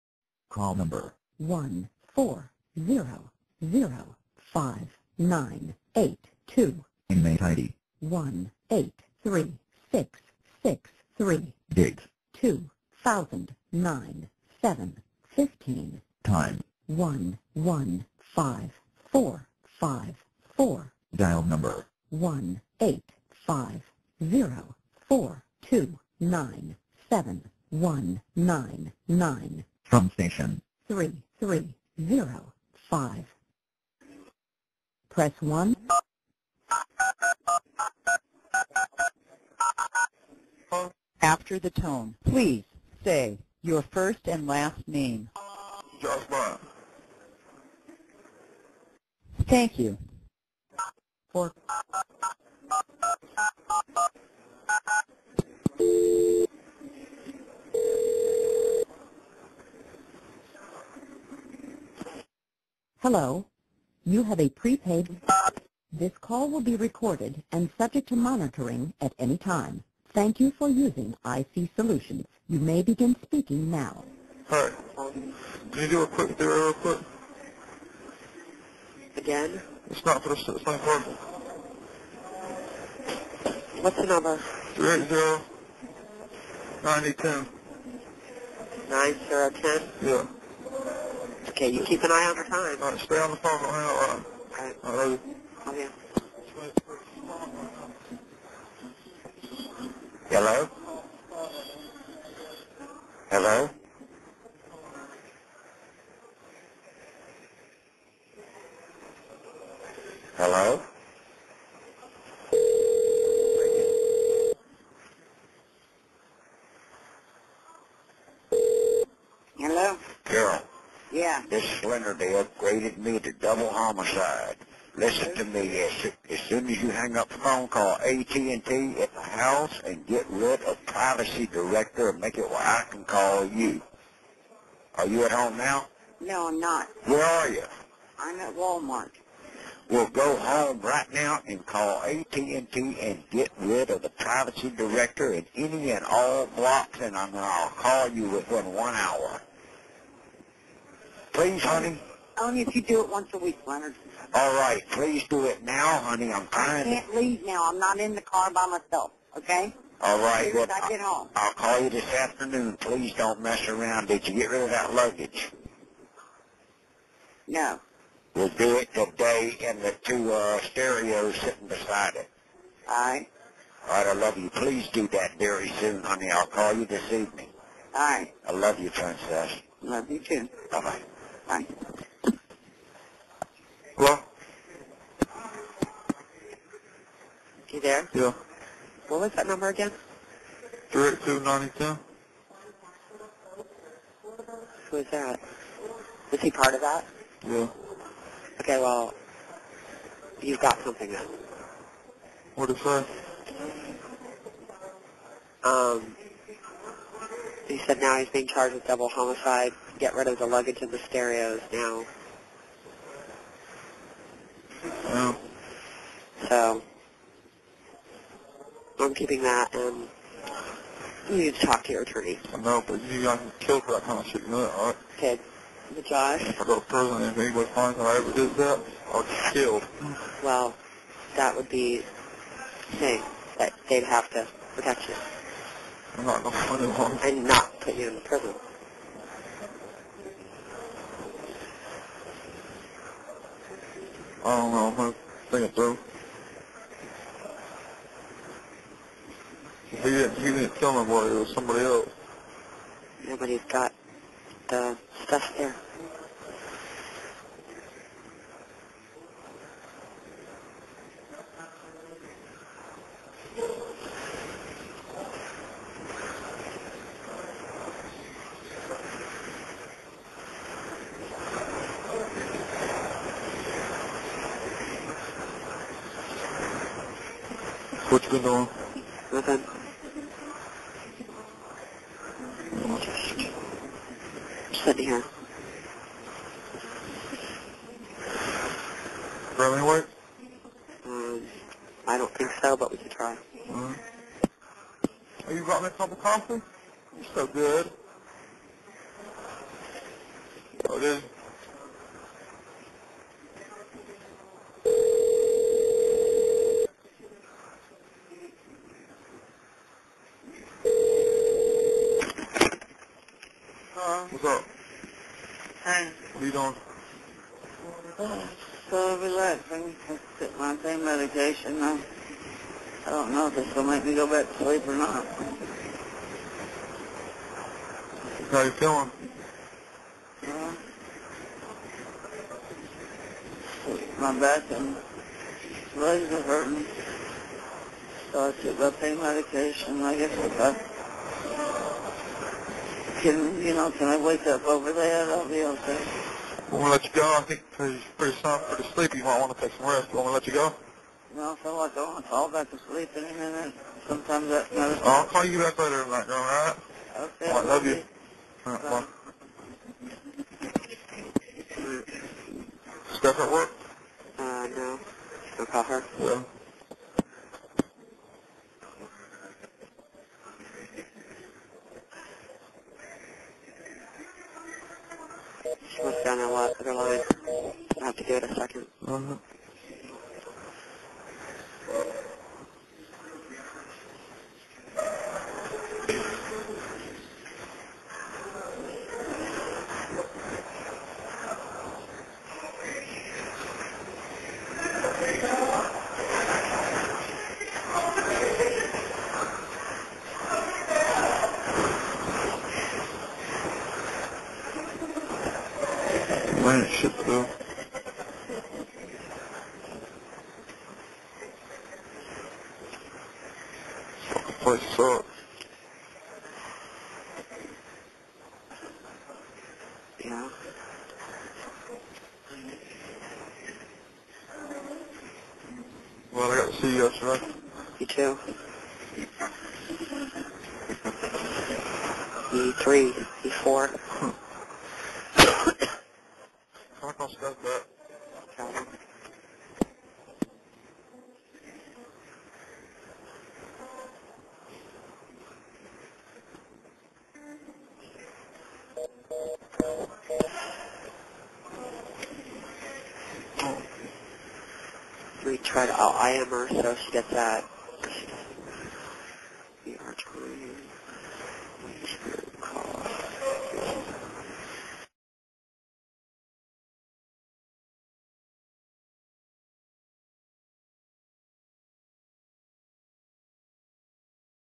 Jail Call 07/15/09 11:54:54am (1 of 2)
Byrd and Melanie Billings Murder Case - Jail Call 07/15/09 11:54:54am Part 1 of 2 - 7:54 min. each), From the Escambia County, Pensacola, Florida public records released through the Florida State Attorney (Bill Eddins) Office, as part of the Florida Sunshine Law, on 10/19/09. For a more extensive archived history of found materials on the Billings murder case, go and register at the acandyrose timeline forum.